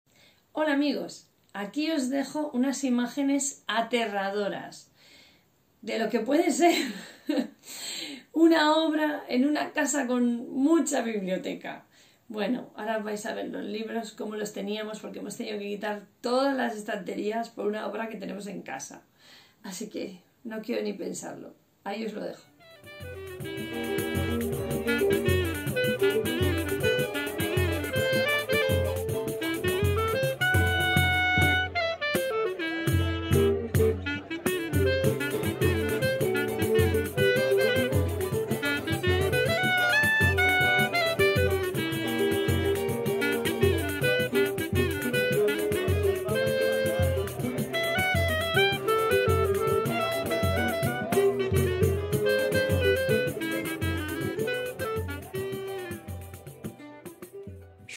¡Estamos de obras con libros amontonados por todos lados!
Un nuevo con las imágenes de nuestros libros por el suelo y los sillones. Estamos de obras con parte de las estanterías desmontadas. Esto es un sinvivir.